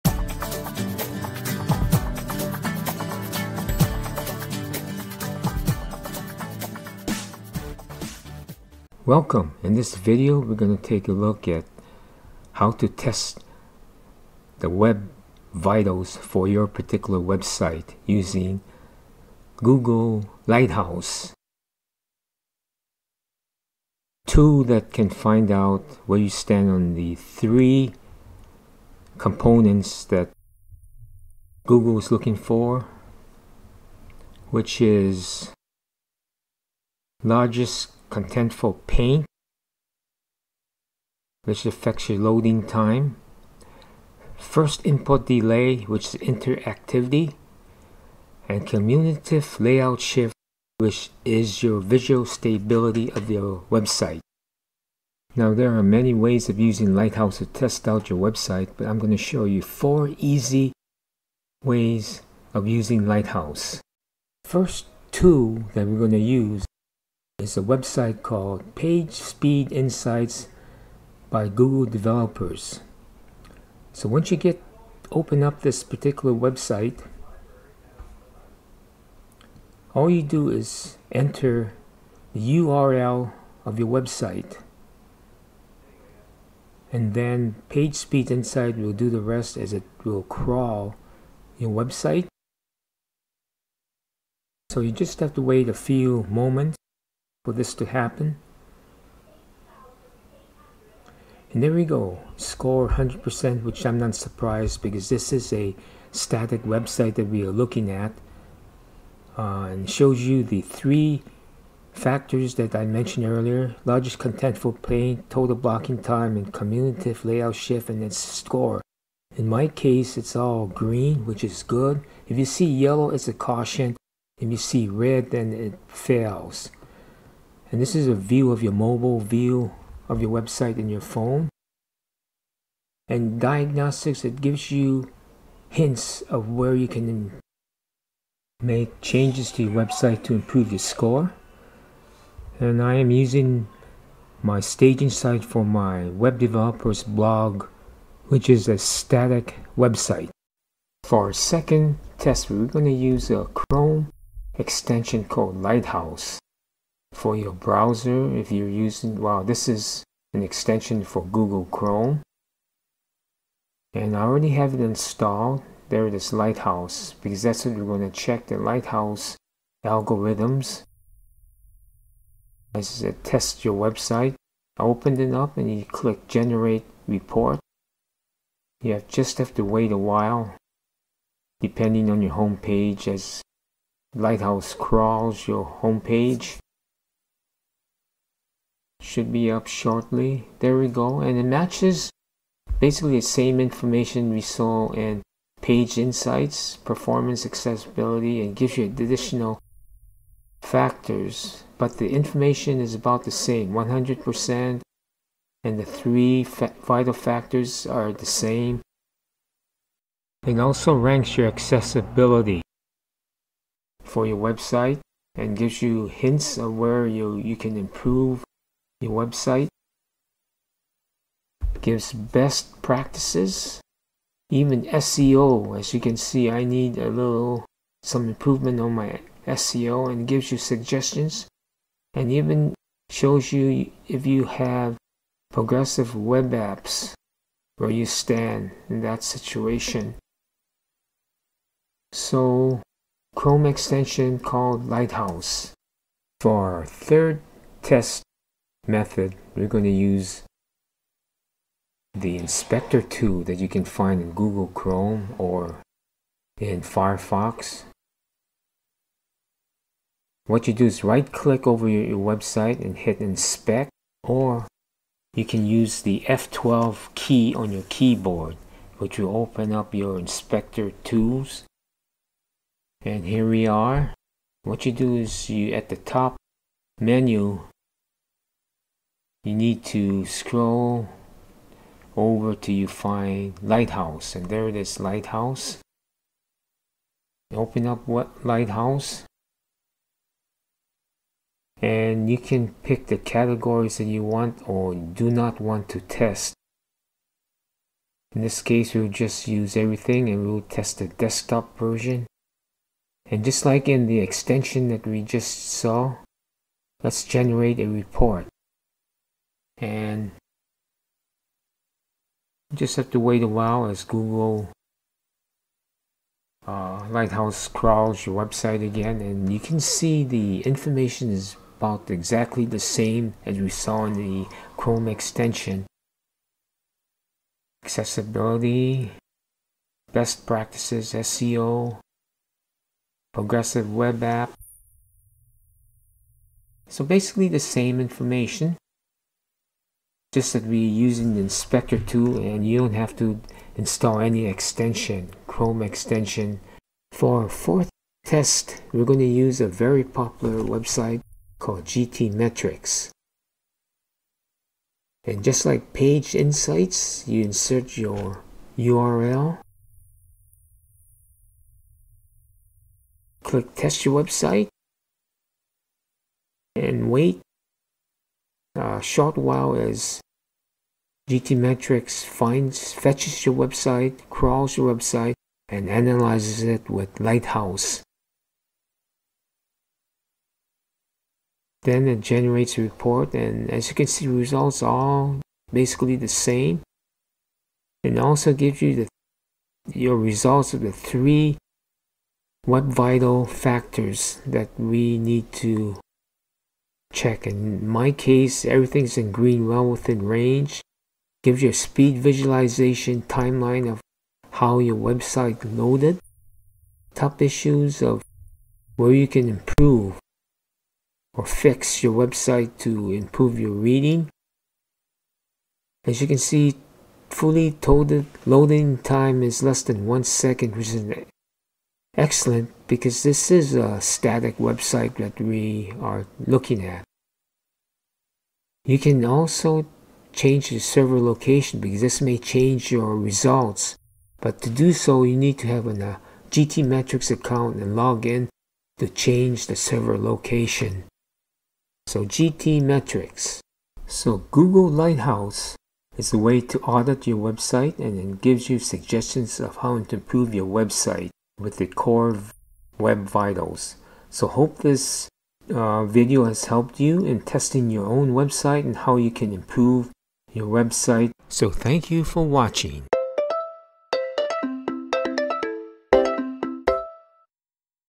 How To Test Your Website Using Lighthouse
Four ways to use Lighthouse to test for Google Web Vitals.

Chrome Inspector Tool